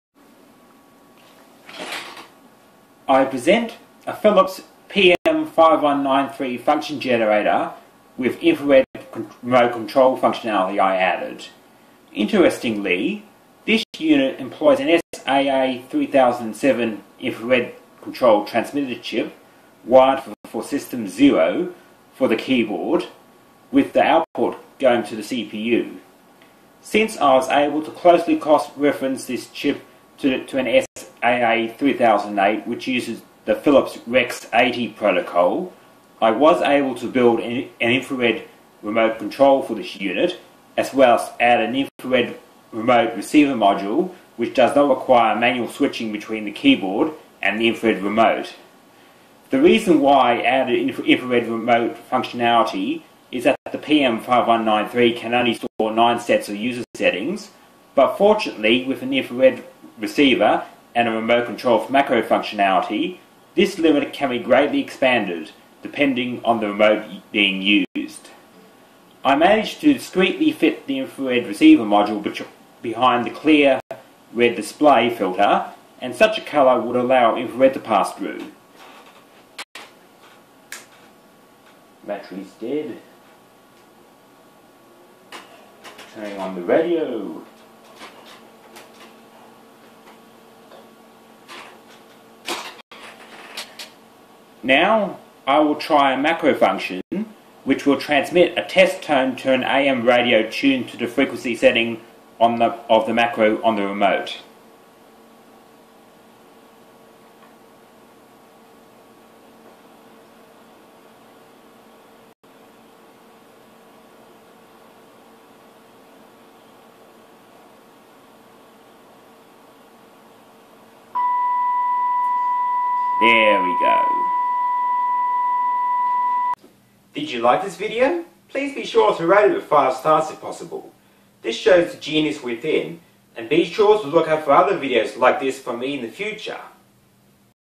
Philips PM5193 function generator with infrared remote control
To overcome the limitation of 9 sets of configuration settings on the Philips PM5193, I added an infrared receiving module and used a universal remote with macro support.
You can find remote control codes for a UEI remote and the hardware modification
Memory 0 can be used as a "neutral" setting, especially for Hz/kHz setting.